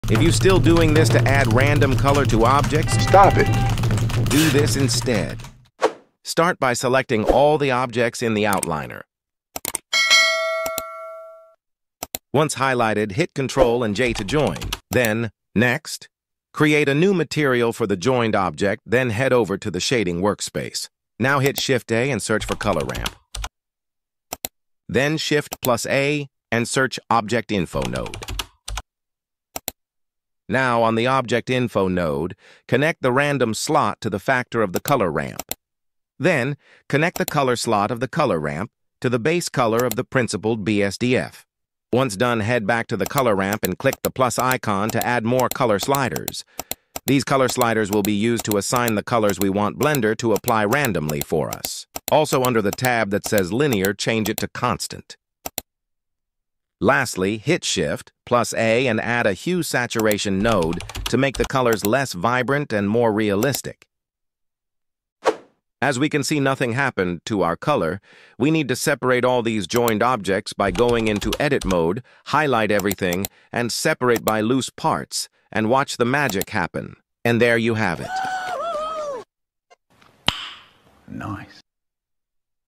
Noob vs Pro artist: Adding Random color
Noob vs Pro artist: adding random color to objects in blender using shader nodes workflow